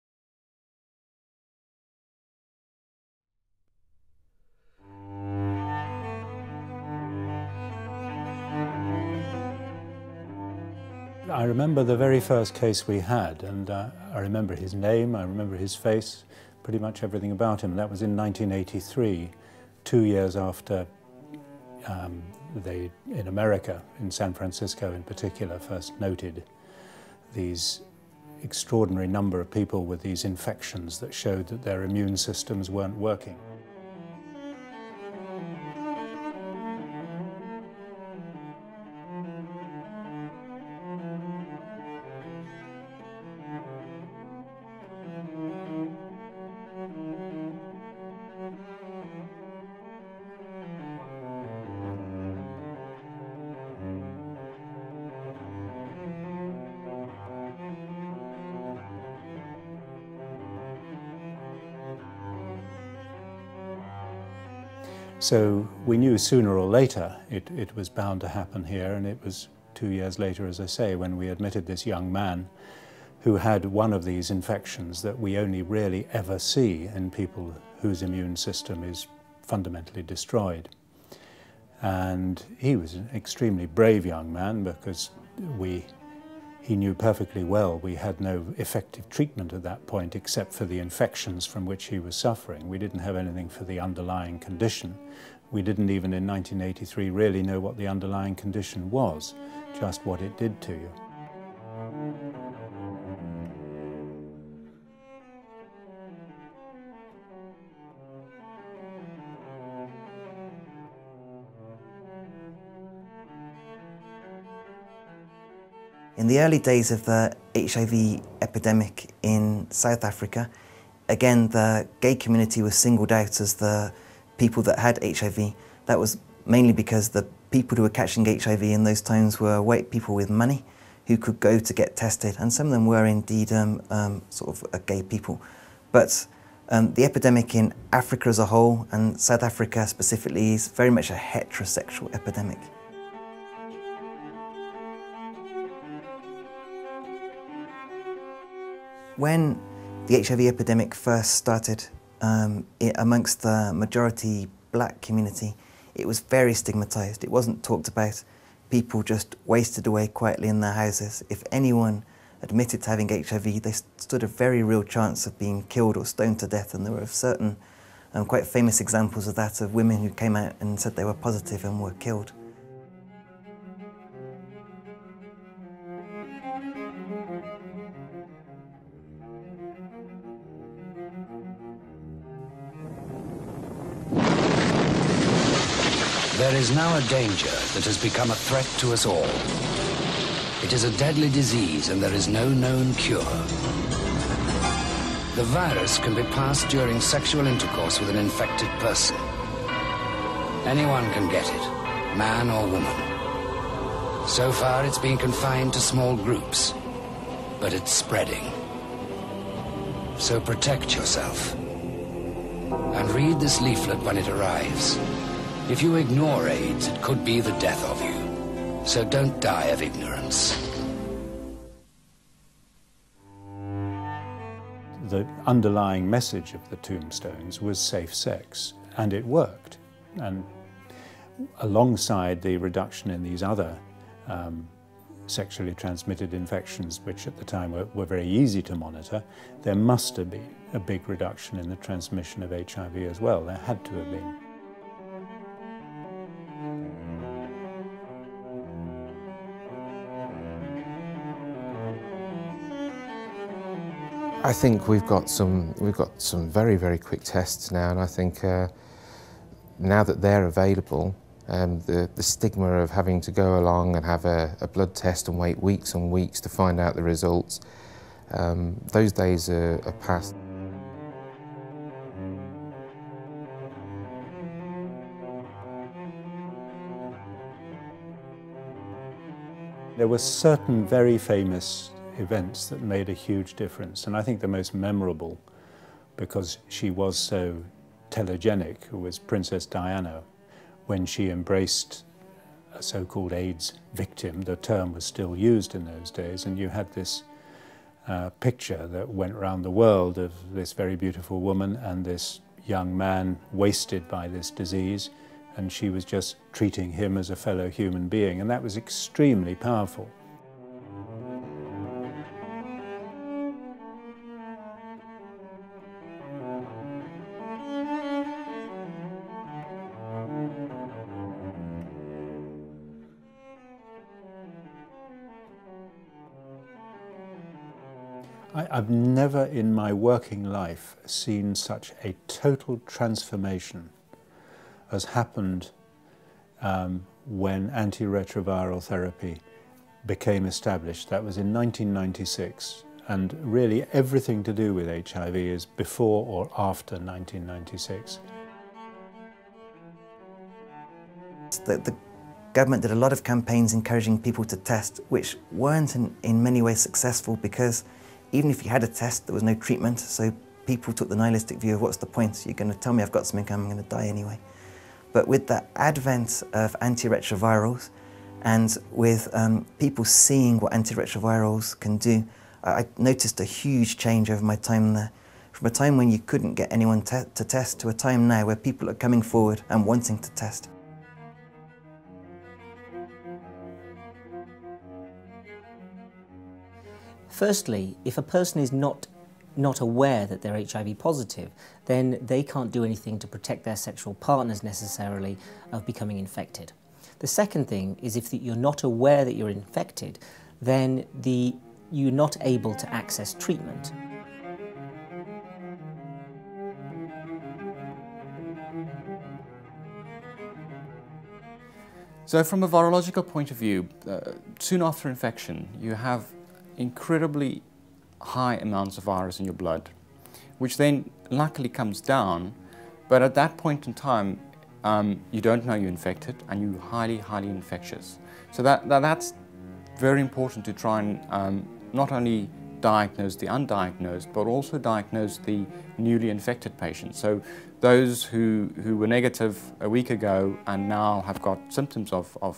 HIV / AIDS 30 Years On (Part 1 of 2)
Part 1: A brief history of the HIV/AIDS epidemic so far. Filmed for the Saving Lives Trust  by Doctors from the Birmingham Heartlands HIV Service.